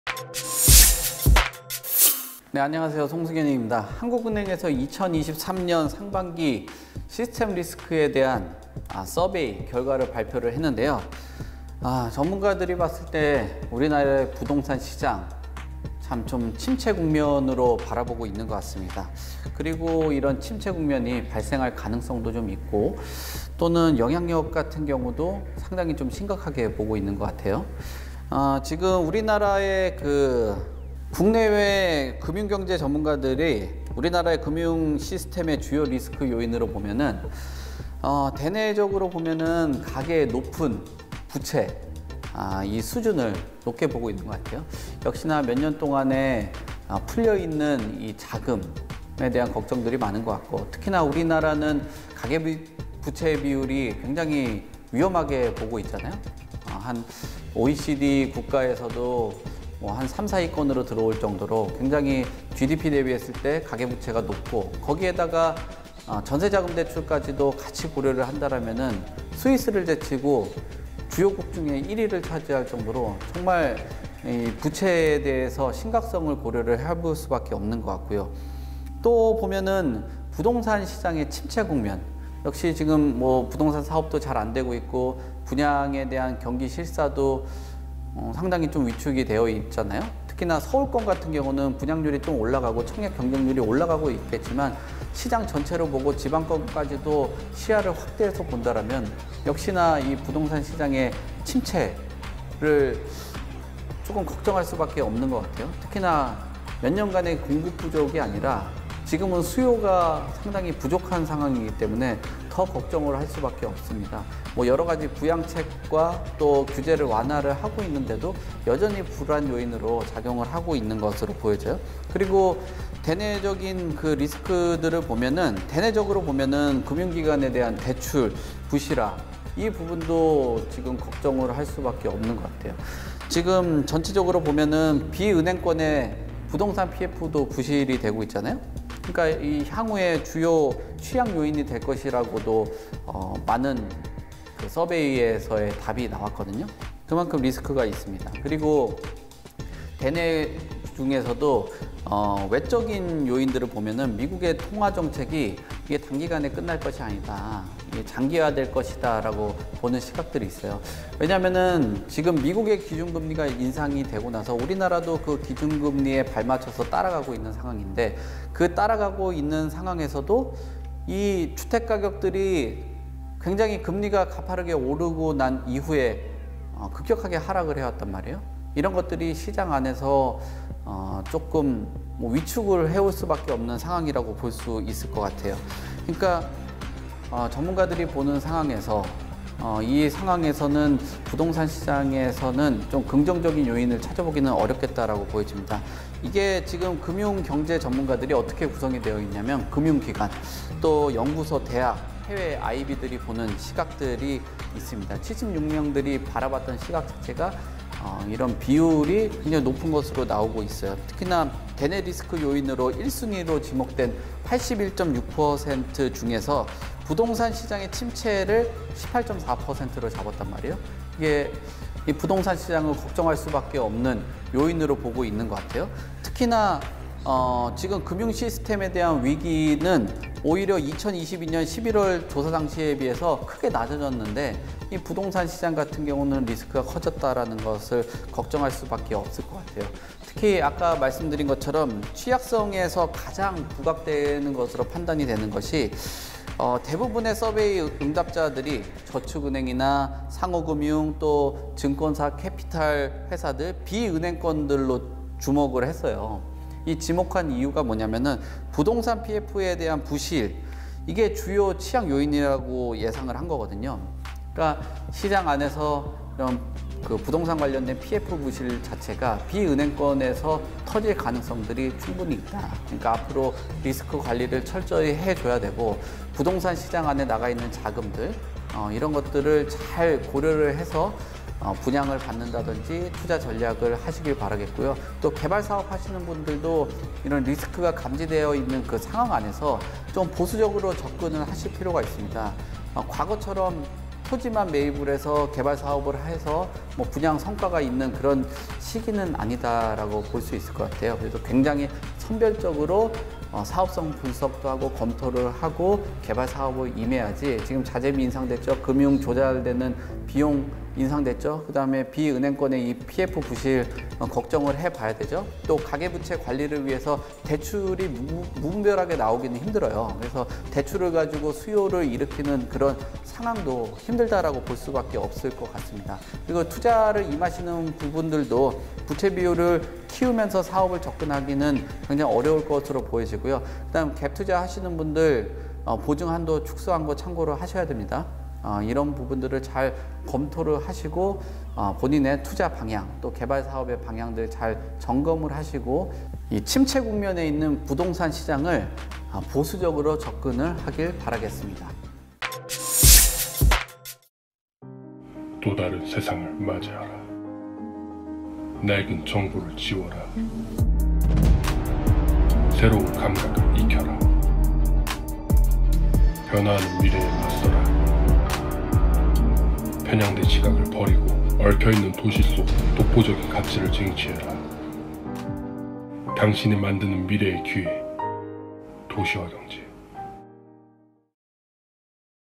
한은, 2023년 상반기 ‘시스템 리스크 서베이’ 결과 발표!! 비은행업권 부동산 PF 부실, 향후 주요 취약요인 될 것 [어반스토리]
[도시와경제]

이번 서베이에서 우리나라 금융시스템의 주요 리스크 요인으로 '가계의 높은 부채 수준 및 상환 부담', '부동산시장 침체', '금융기관 대출 부실화 및 우발채무 현실화, 대규모 자금인출 가능성' 등으로 조사

단순 응답빈도수 기준 대내 리스크 요인
'가계의 높은 부채 수준 및 상환 부담 증가'(53.9%), '부동산시장 침체'(48.7%), '금융기관 대출 부실화 및 우발채무 현실화, 대규모 자금인출 가능성'(43.4%) 등을, 대외 리스크 요인으로는 '미국의 통화정책 긴축 장기화'(28.9%) 등을 지목

대내 리스크 요인
1순위로 지목(81.6%)한 가운데 '부동산시장 침체'(18.4%), '기업의 업황 및 자금조달 여건 악화에 따른 부실위험 증가'(13.2%), '국내 금융·외환시장 변동성 확대'(10.5%) 등을 주요 리스크로 선정

기업 부실위험, 금융기관 대출부실화, 국내 금융·외환 시장변동성, 경상수지 적자, 부동산시장 침체 등 가계부채를 제외한 주요 리스크는 주로 단기(1년 이내)에 가계부채와 관련된 리스크는 중기(1~3년)에 위험이 현재화될 가능성이 높다고 판단하는 것으로 조사